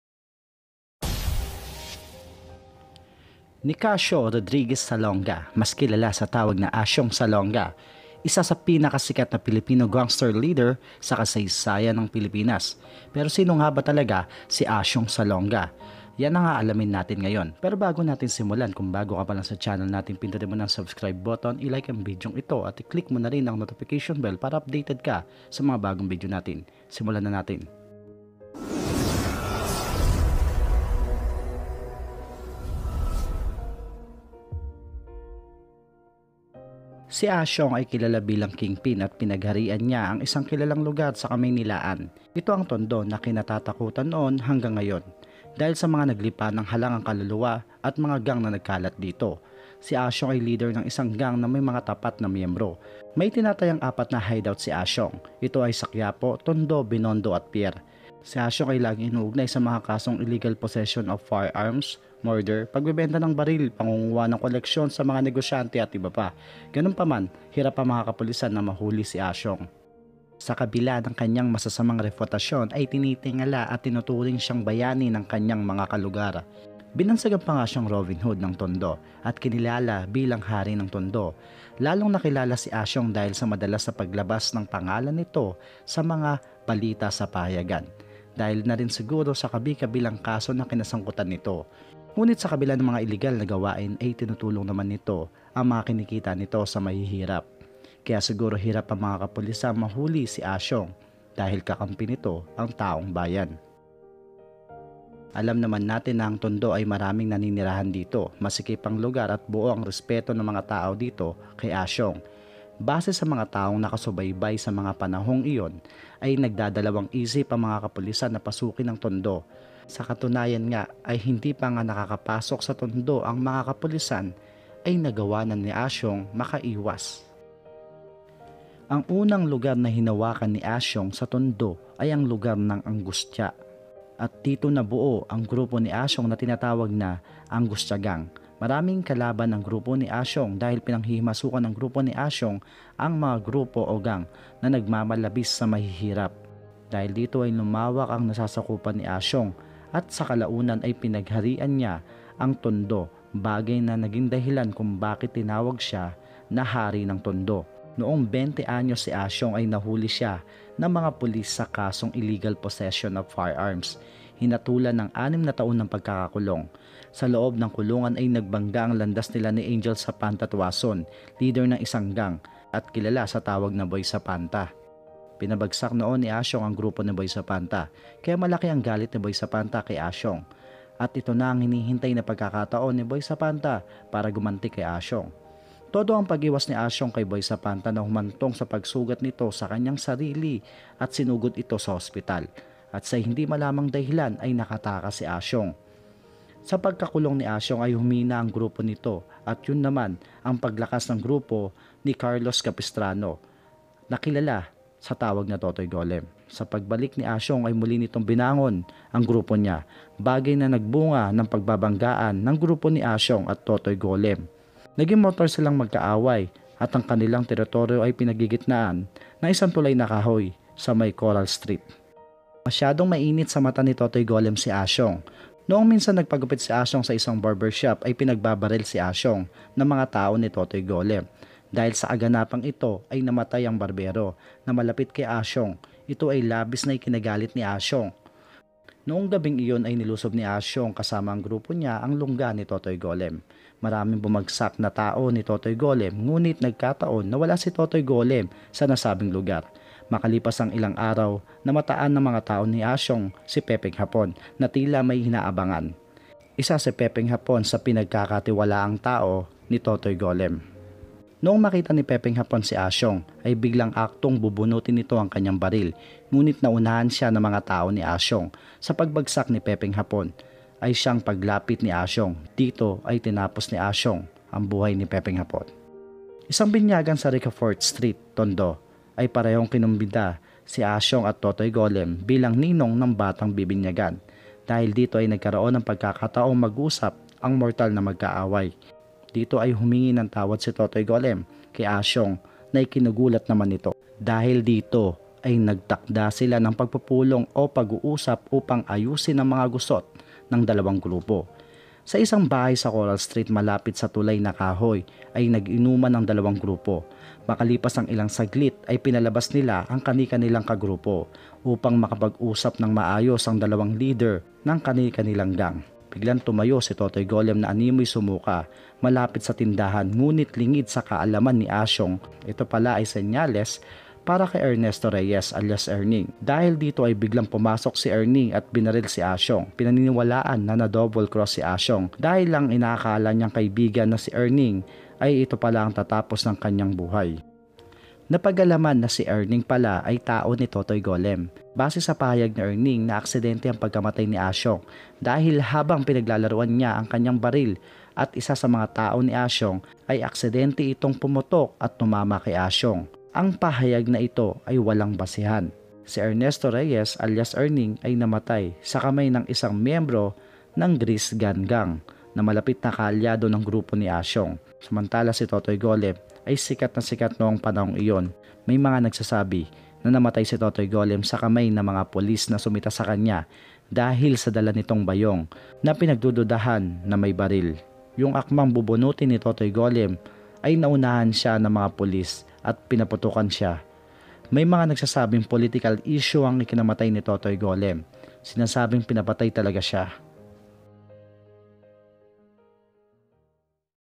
Asiong Salonga Story
Sino nga ba si Asiong Salonga ang tinaguriang Hari ng Tondo